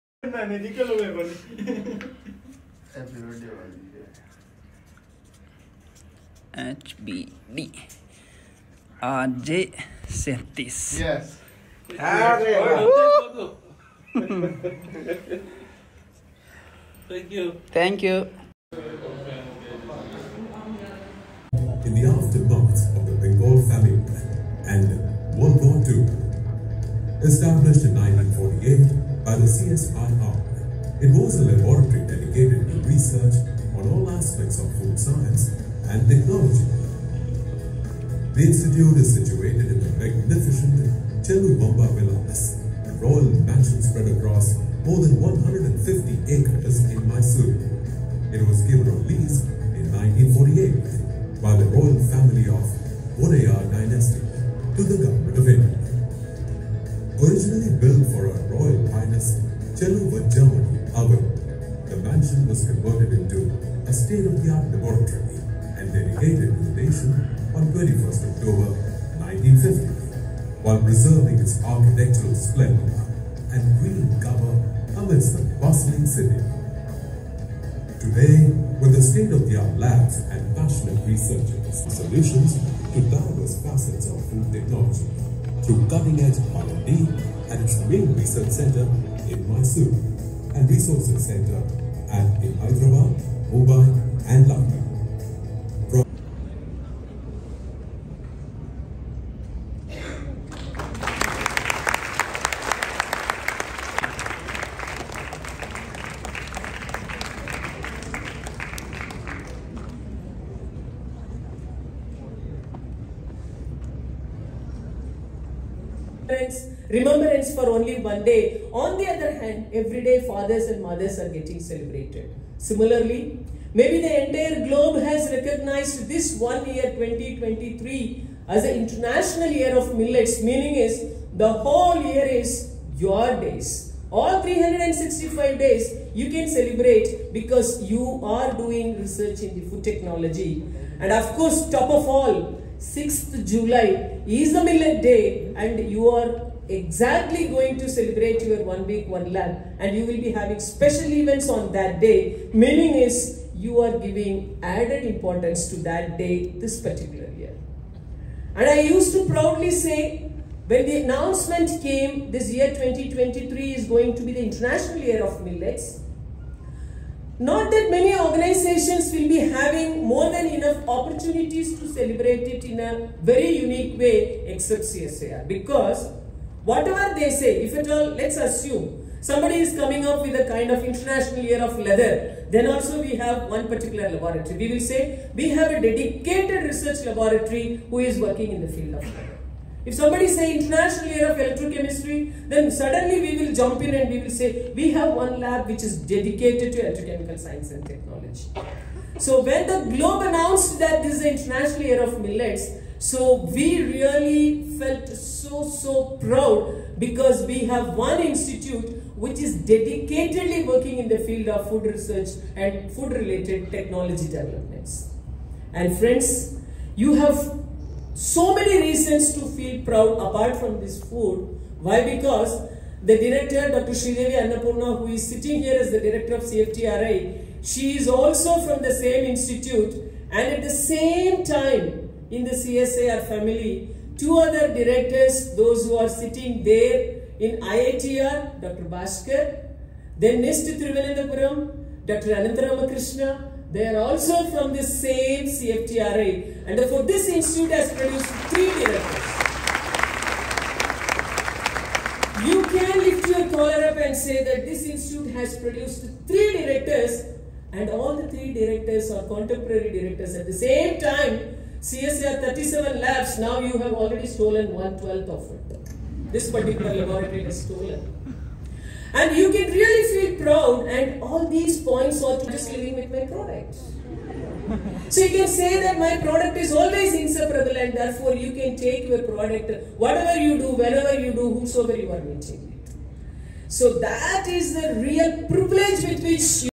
One week lab program || Organized by CFTRI
After finishing our trip , we got to know that our CFTRI is organizing one week lab program for enthusiastic candidates who wanna to go through all researches and get a opportunity to study in CFTRI